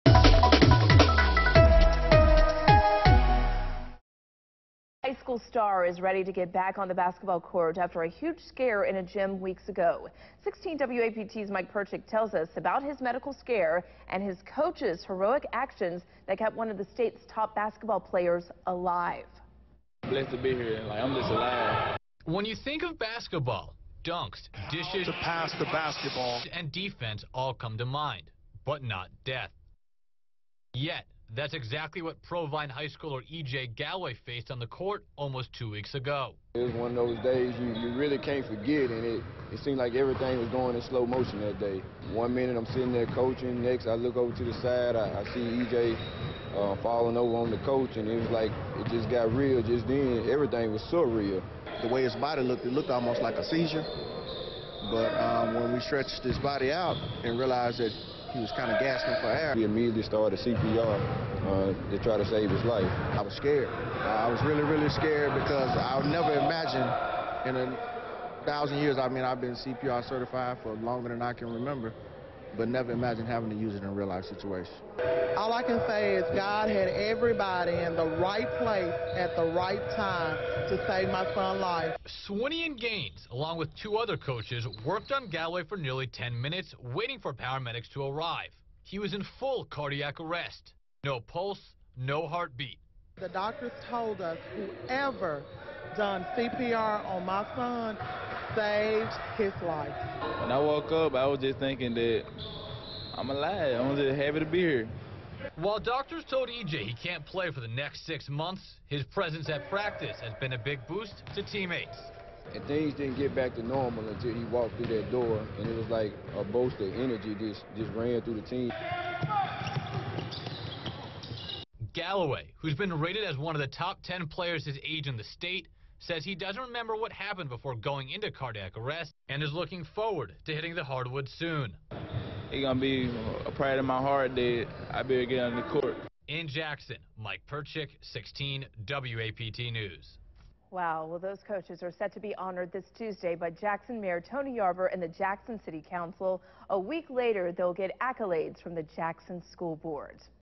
Provine basketball player returns to practice after being revived from cardiac arrest
A Provine High School basketball player returns to practice after his coach revived him using CPR after he suffered cardiac arrest two weeks ago.